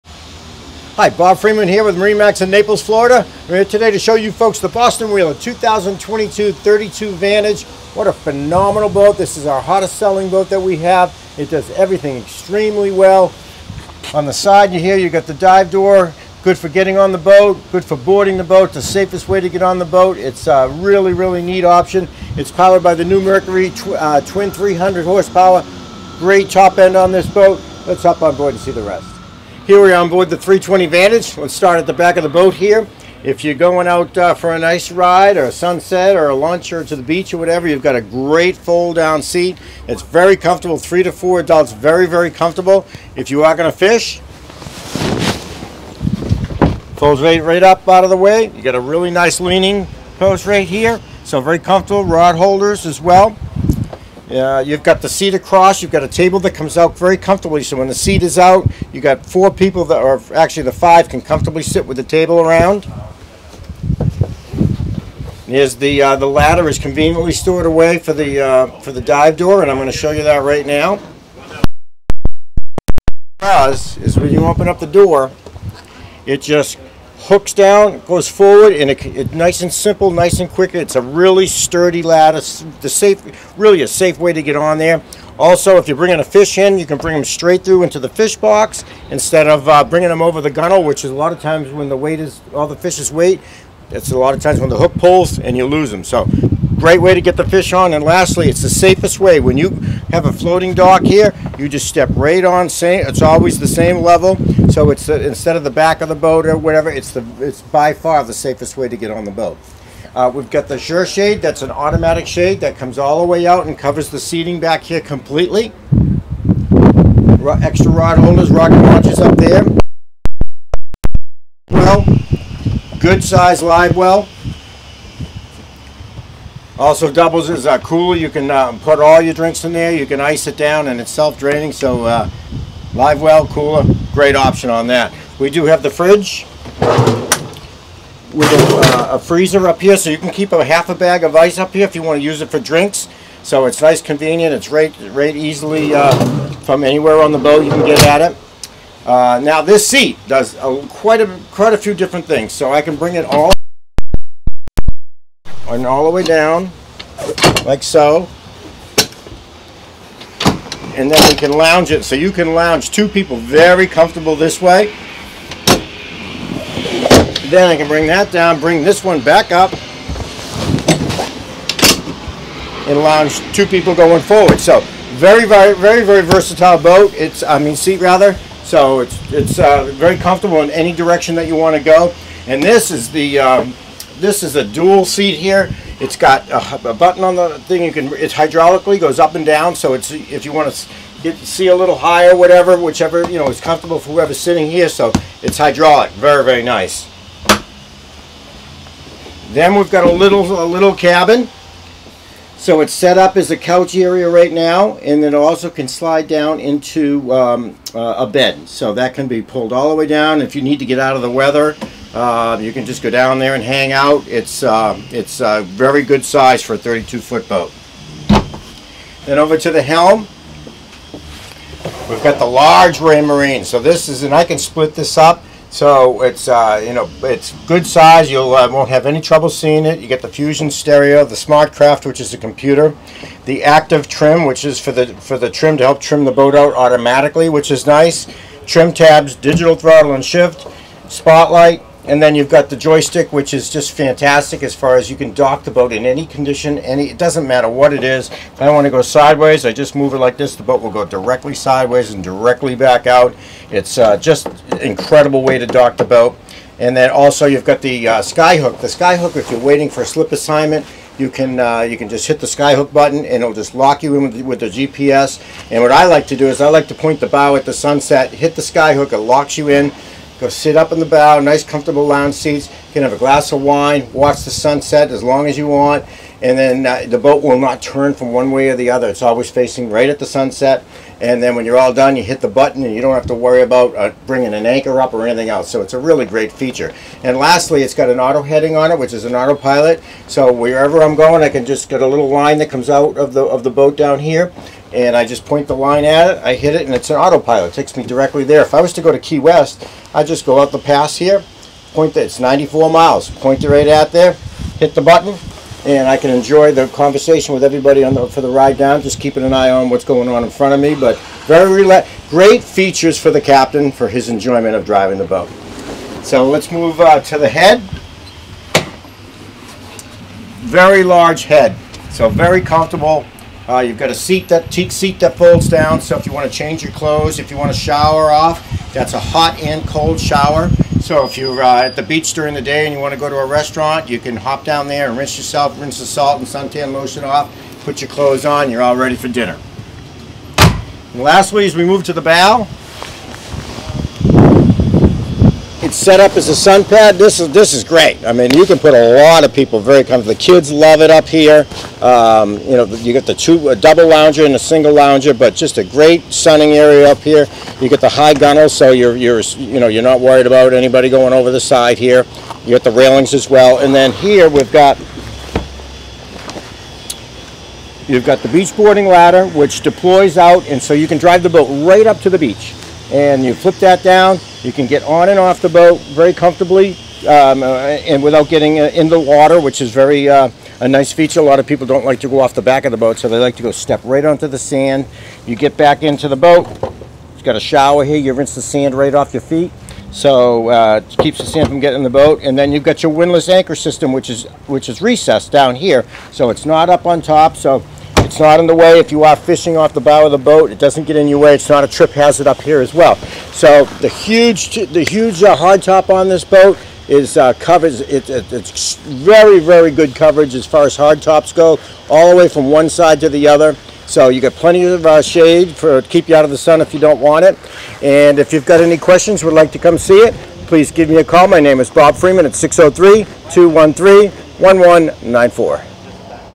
2022 Boston Whaler 320 Vantage For Sale at MarineMax Naples Yacht Center
Cutting-edge features, great capacity and easy adaptability are hallmarks of the dual-console 320 Vantage®, winner of the National Marine Manufacturers Association’s Innovation Award. Rock-solid control meets the ultimate in versatility and comfort with the many accessories and options the 320 provides. Serious fishing, joyful entertaining, day-long watersports or cruising and even a place to sneak in a nap or an overnight stay–the 320 is ready for your day. When the softest, safest ride meets the best equipped dual-console around, new possibilities become eminently possible. With Whaler, you can be confident to the core.